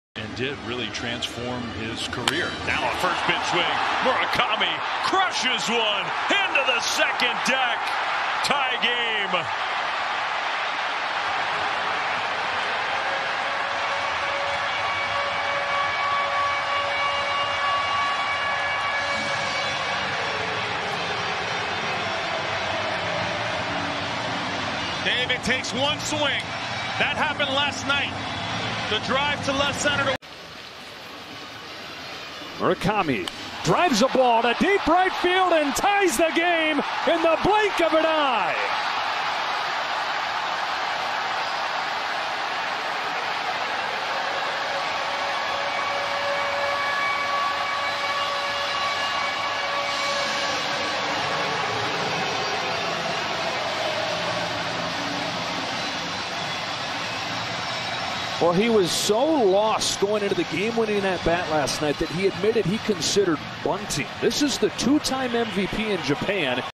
W.B.C. 2023 Championship Game: Munetaka Murakami Home Run (World Feed Version & FS1 Version)
Calls from Dave Flemming & Joe Davis on a Solo Home Run by Munetaka Murakami.